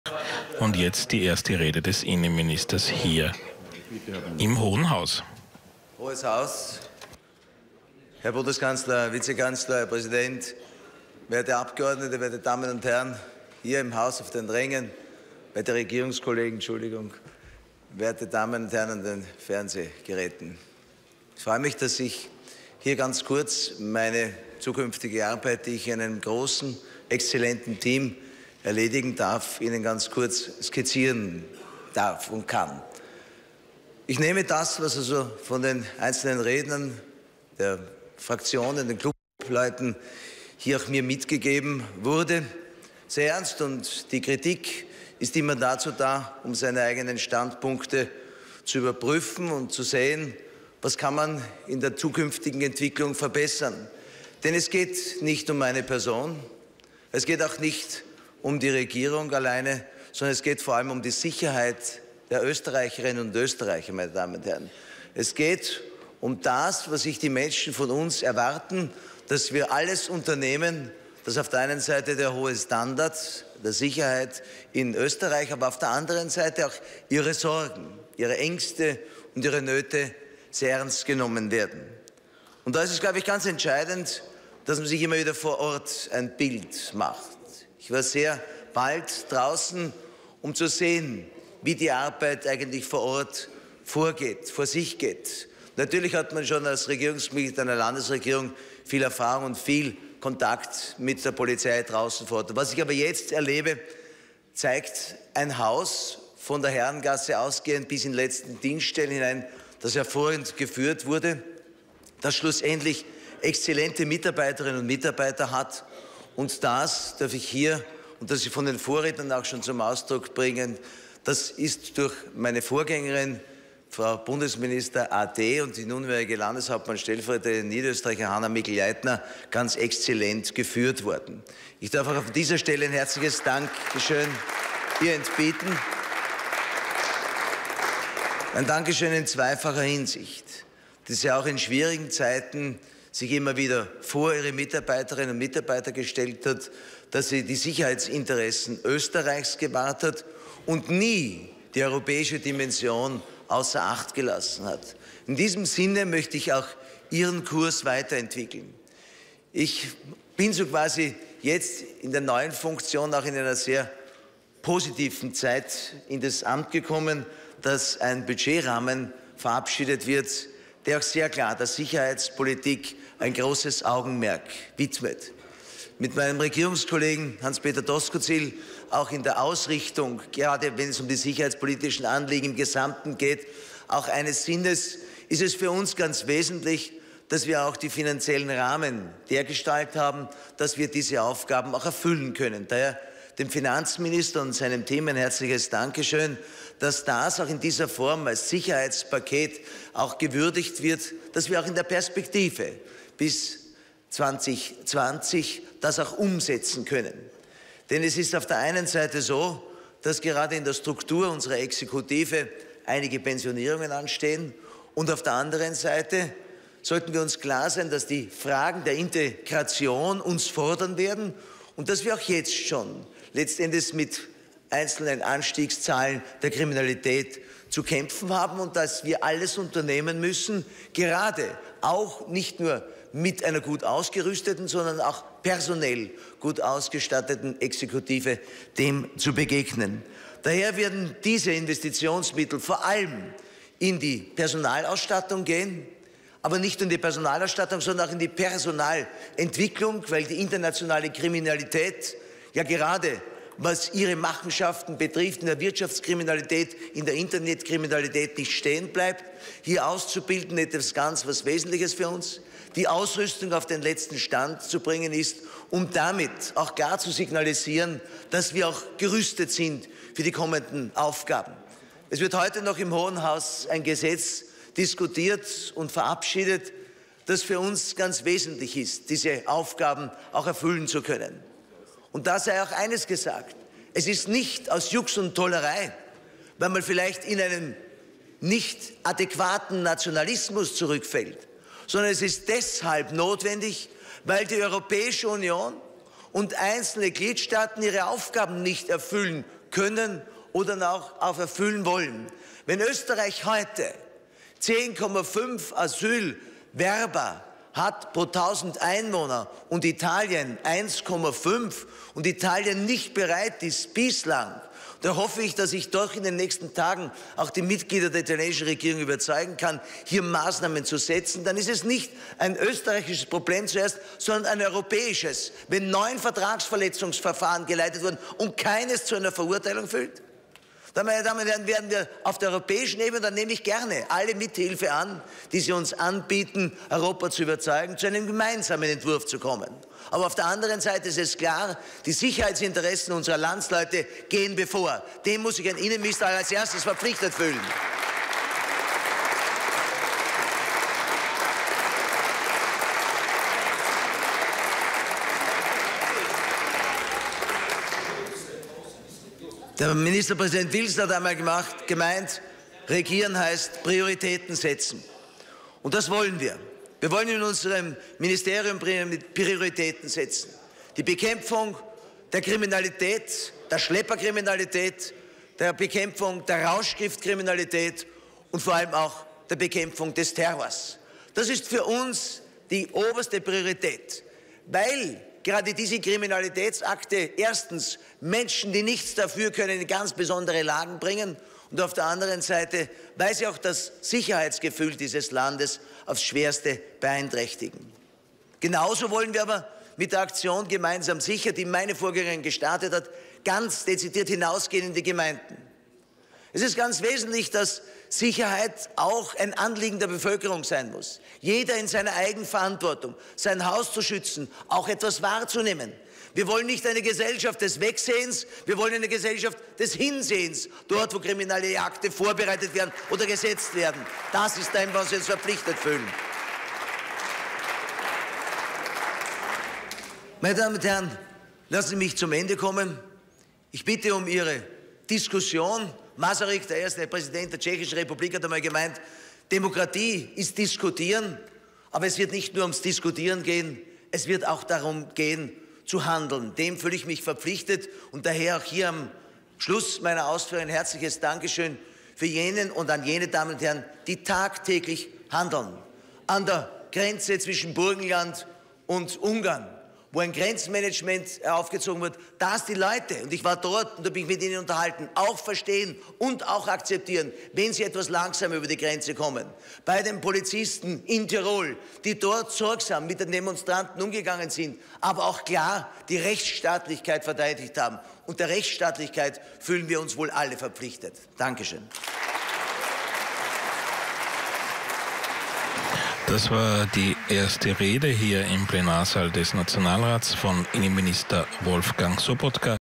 Antrittsrede von Innenminister Wolfgang Sobotka im Nationalrat 27.4.
Innenminister Mag. Wolfgang Sobotka hat im Nationalrat seine Antrittsrede gehalten und dabei die Schwerpunkte der nächsten Monate präsentiert.

(C)ORF TVThek